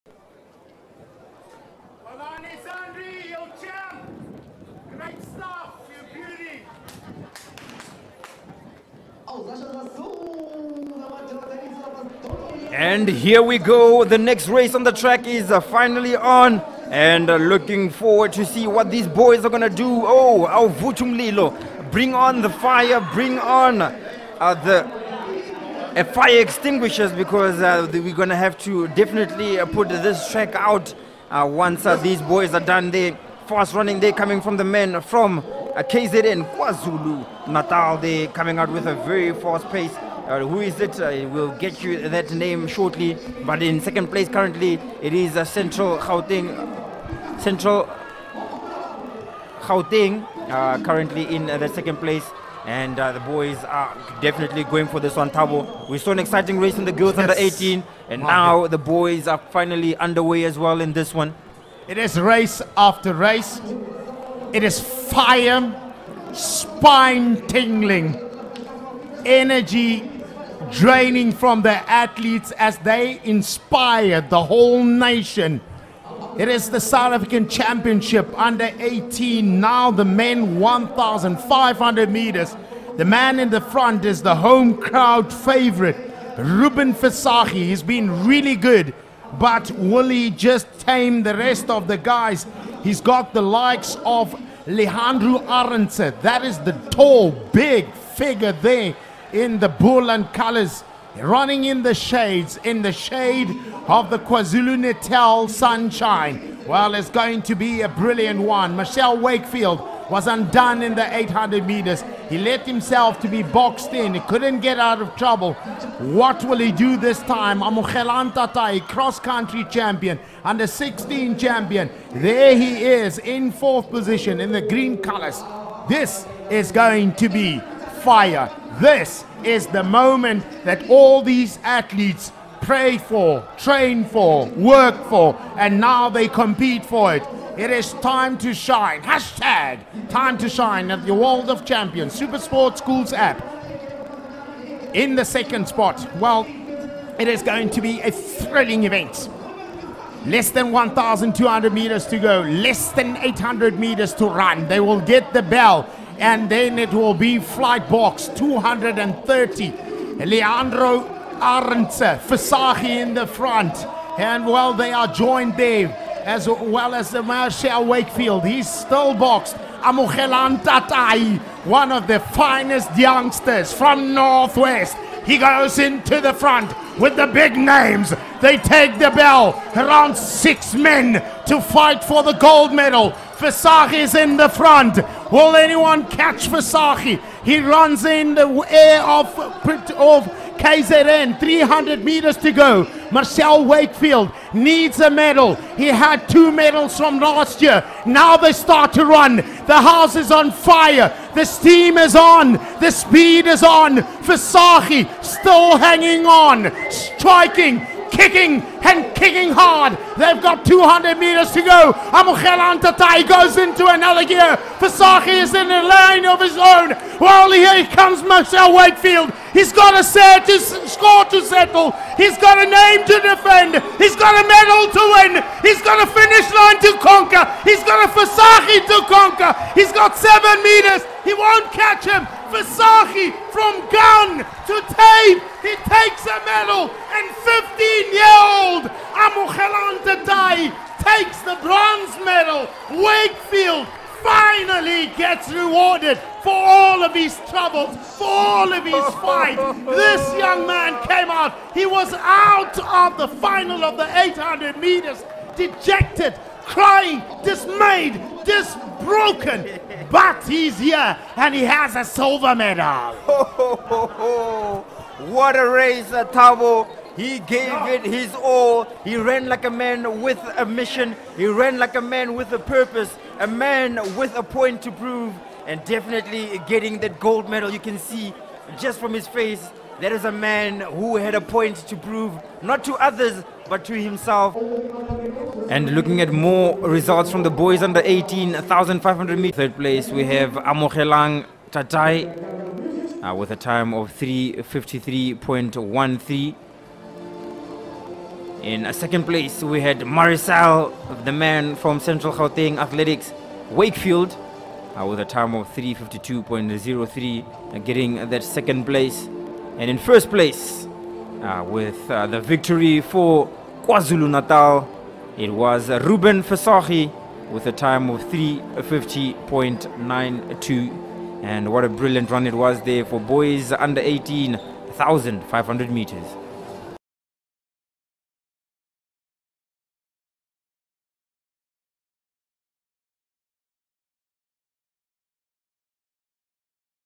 Ruben Visagie wins 2023 SA Boys U18 Championships 1500m in 3:50.92 in Pietermaritzburg
2023 South African U16, U18 & U20 Championships in Pietermaritzburg - Day 2 (24th March)

1500m Boys U18 Final

1     Ruben VISAGIE                             KZNA    3:50.92
2     Marceghlle WAKEFIELD              CGA      3:52.03
3     Amogelang TATAI                        ACNW  3:53.13
4     Marcel BRITS                                AGN      3:54.58
5     Xavier VAN DER LEEK                 AGN      3:58.45
6     Lee-Hanro ARENDSE                   BOLA    3:59.44
7     Matthews MACUACUA               AGN.     4:00.87
8     Rico COETZER                             AGNI     4:02.67
9     Realeboga SMITH                       CGA      4:04.10
10   Tshepang TSHIVULA                  AGNI    4:04.42
11   Jack MALAHLELA                      LIMA     4:05.15
12   Oratile MASHIGO                        CGA.     4:07.62

Video Credit: SuperSport Schools (Pty) Ltd
Video Source: SuperSport Schools App